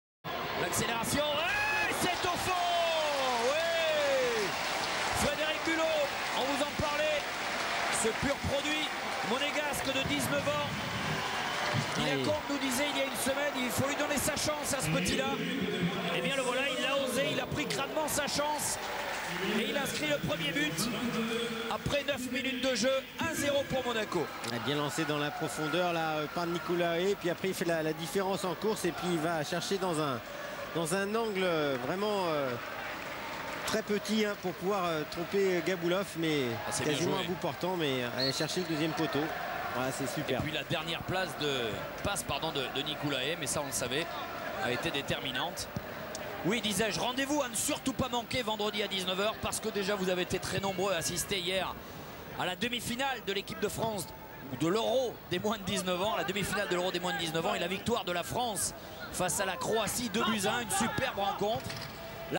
As Monaco 1-0 Dynamo Moscou(Frédéric Bulot)28.07.10
28 июля 2010, Монако, стадион "Луи II", товарищеский матч.